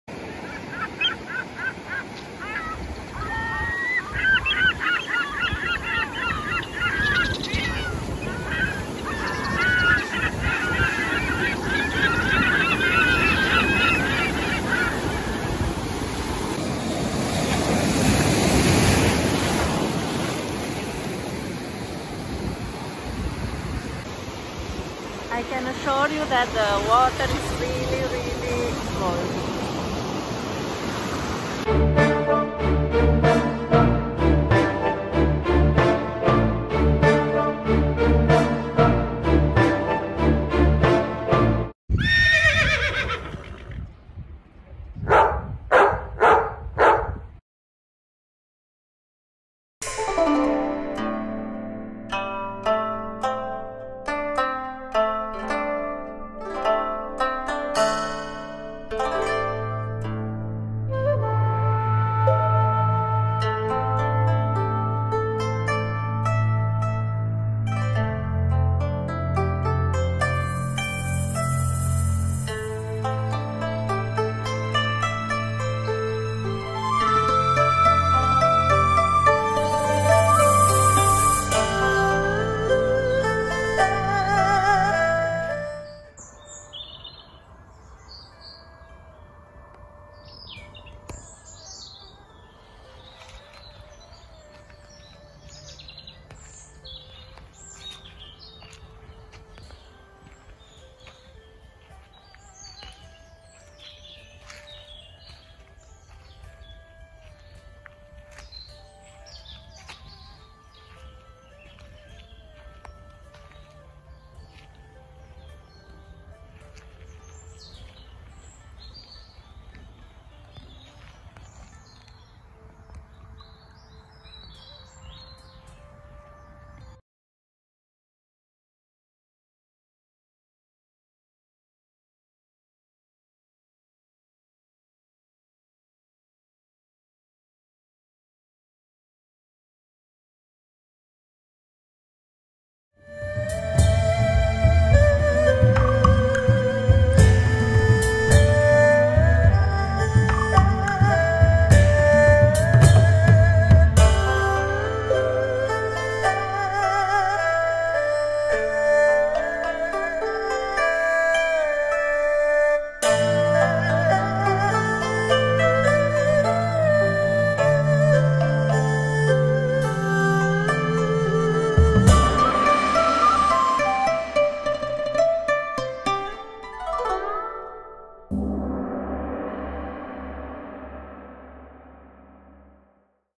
Me enjoying the beach and with Aris the dune forest at De Haan, on Belgian Coast in a sunny day
De Haan has a beautiful wide sand beach beach. It is the longest of the Flemish coast, of about twelve kilometres in length and 50-80 meters wide. To the west, you will find the only nudist beach of Flemish coast. In the summer, the beach is pleasantly busy, but not too crowded. The beach is rather quiet not like Ostend Beach.

Duinbossen (the dune forest) in De Haan is like a green oasis on the Belgian Coast. The area consists of three parts and covers more than 152 hectares. You can enjoy the wonderful combination of an open dune landscape, with its typical fauna and flora, and vast forestation. Of course with Aris I needed to pay attention to the dogs walking there. This forest is a paradise for them together with the beach.

Aris doesn't like the beach, only the forest and the parks where he can hide and feel safe.